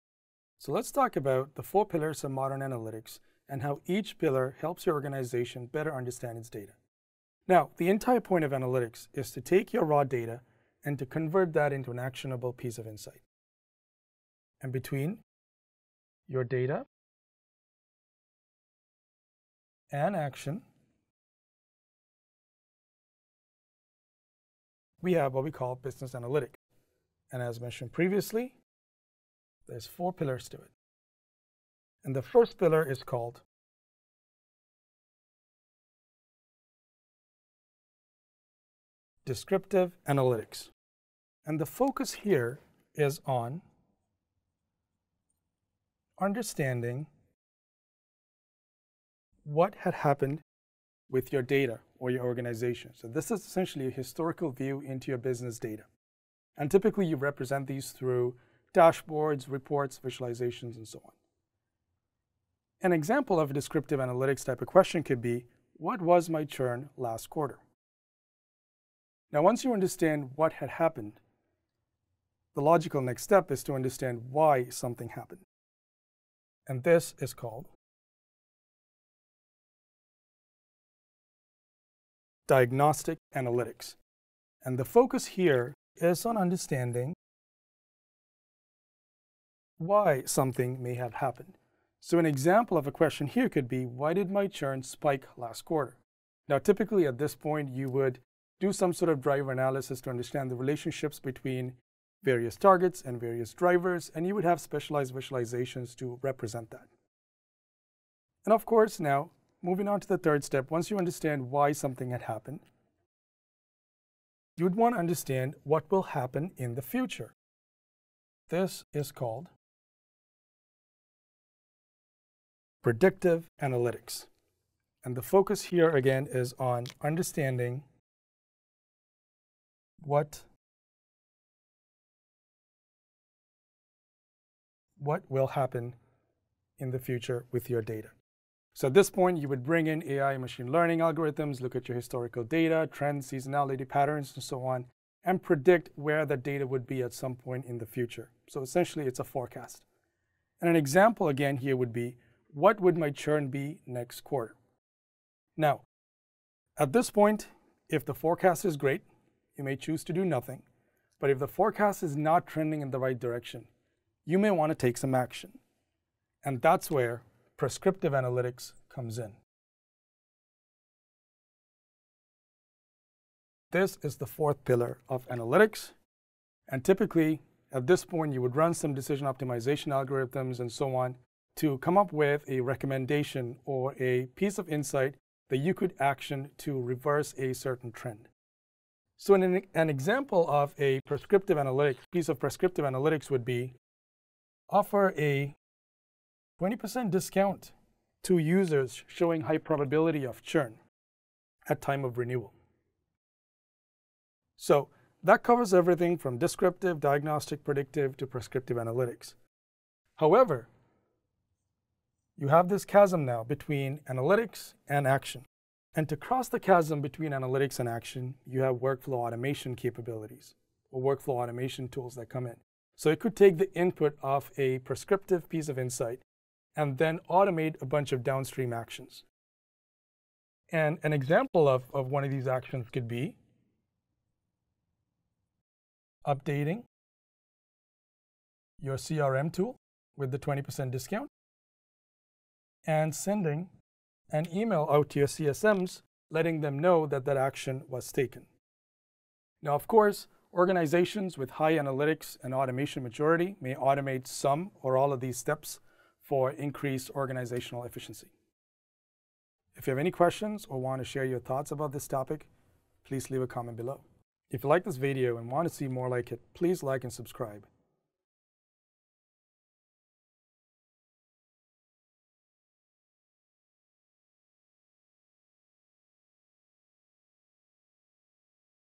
The 4 Pillars of Core Analytics
Want to play with the technology yourself? Explore our interactive demo

In this video, Kas Attanapola explains how descriptive analytics provides a historical view, diagnostic analytics uncovers reasons behind trends, predictive analytics forecasts future outcomes, and prescriptive analytics offers recommendations for strategic actions - and how workflow automation bridges the gap between analytics and action for increased organizational efficiency.

00:31 - Descriptive Analytics
01:09 - Diagnostic Analytics
01:47 - Predictive Analytics
02:34 - Prescriptive Analytics
03:29 - Bridging the gap between analytics and action

AI news moves fast. Sign up for a monthly newsletter for AI updates from IBM